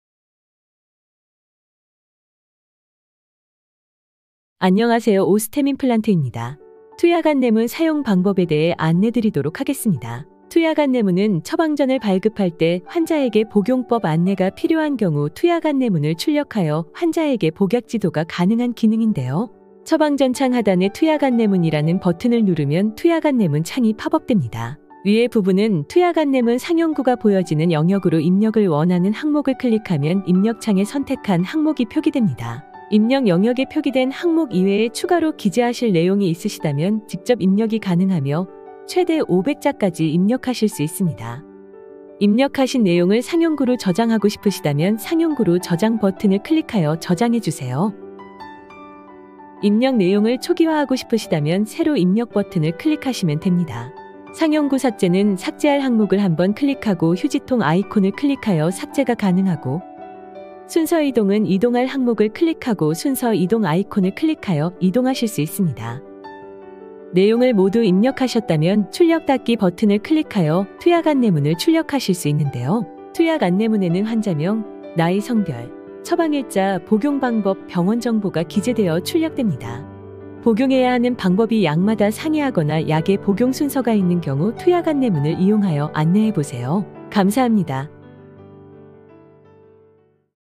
[OneClick] 투약안내문 출력하기
▶ 투약안내문 출력하기
① 원외처방전의 [투약안내문] 버튼을 클릭하여 투약안내문 창을 띄웁니다.

② 투약 안내문 상단에는 투약문 상용구가 보여집니다.

③ 투약 안내문 리스트 선택 시 삭제 아이콘이 생성되며, 아이콘을 클릭하여 항목을 삭제할 수 있습니다.

④ 투약 안내문 하단에는 내역을 입력할 수 있으며, 최대 500자까지 입력 가능합니다.
⑤ [새로입력] 버튼 클릭 시 입력창에 작성된 내용이 삭제되며, 입력창이 공란으로 제공됩니다.

⑥ [상용구로 저장] 버튼 클릭 시 입력창에 입력한 내용을 상용구로 추가할 수 있습니다.

⑦ [출력/닫기] 버튼을 클릭하여 입력한 투약 안내문을 출력할 수 있습니다.